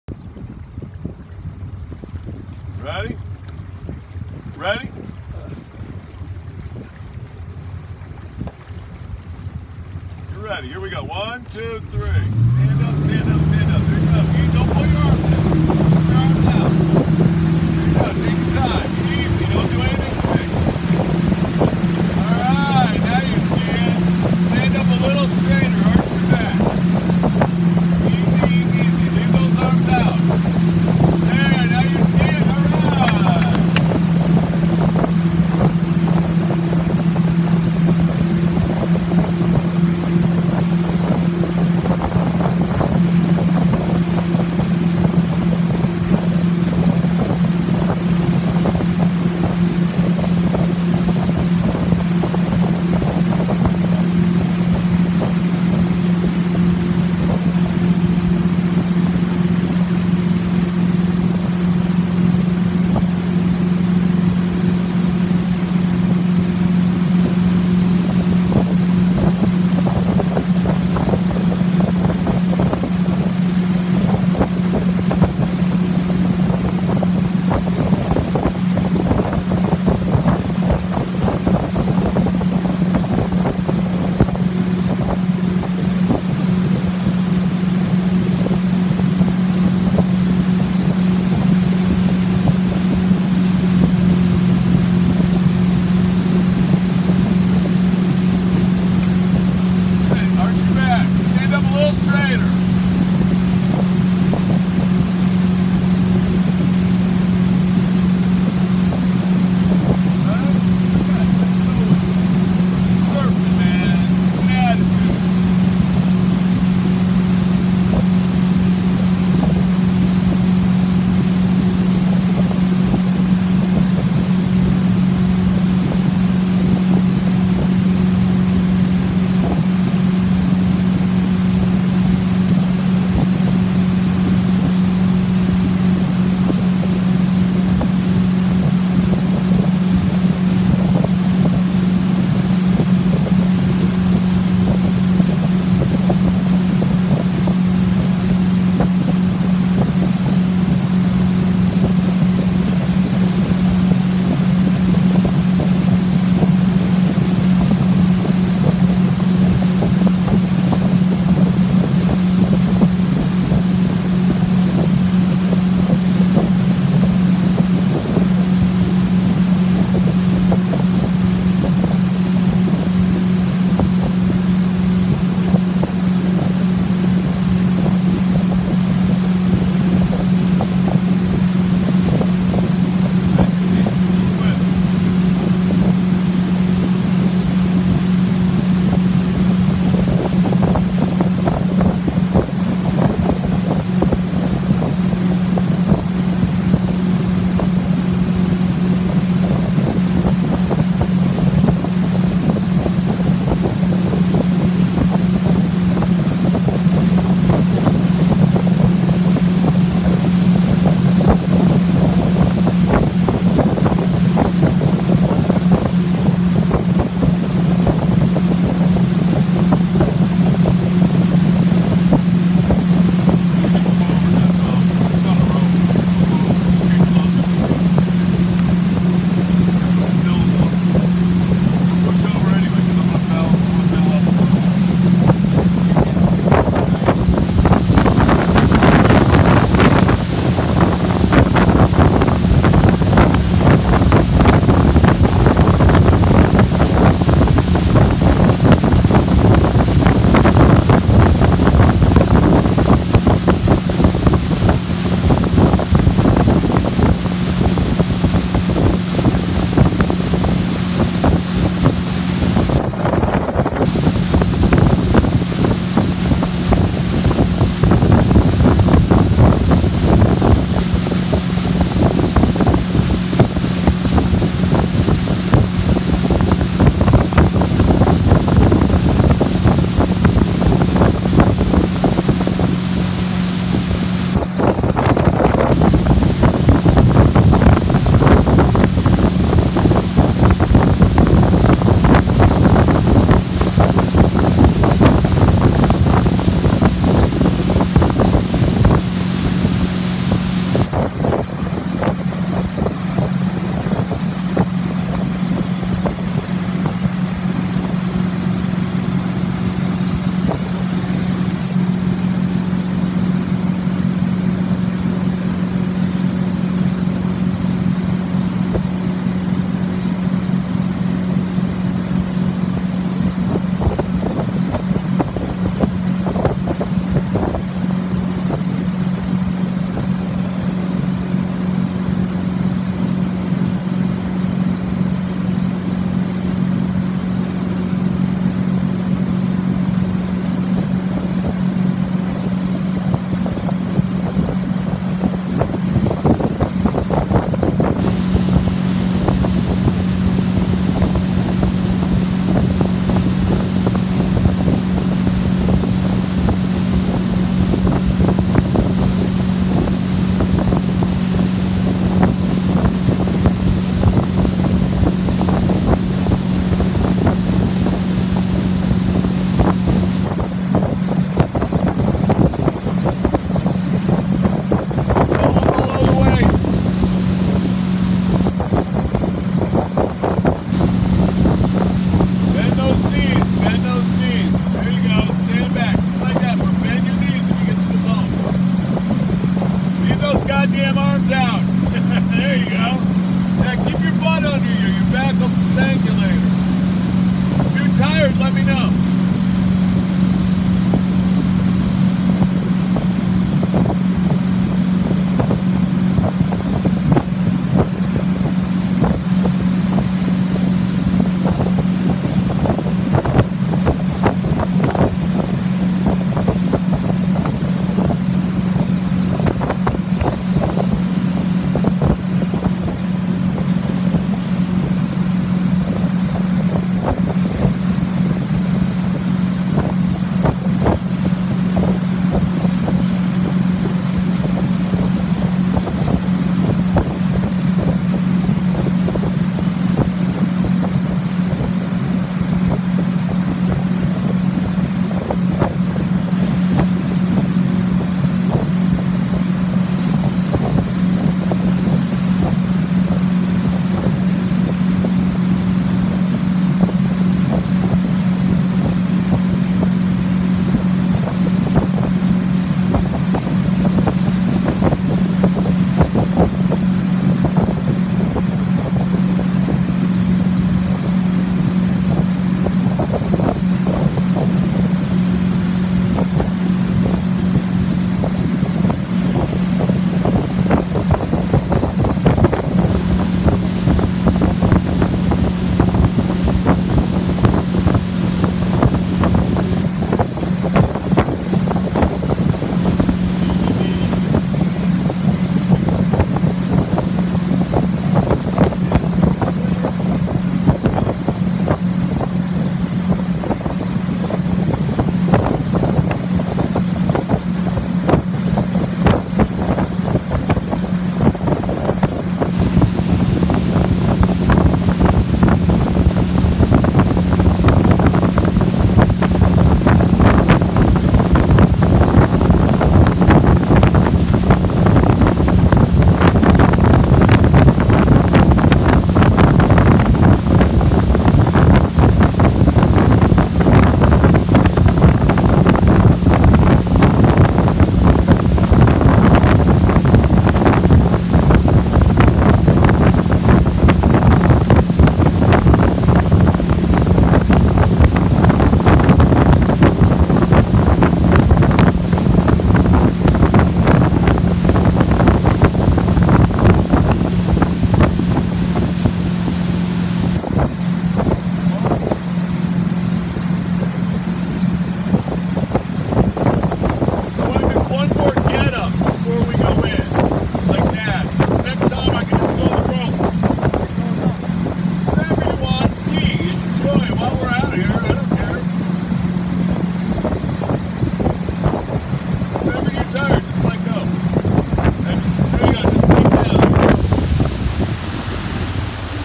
Water skiing on Lake Mahopac.
Lake Mahopac is very picturesque in the Summer.  This is a longer clip of me skiing around the beautiful scenic lake under the warm Sun.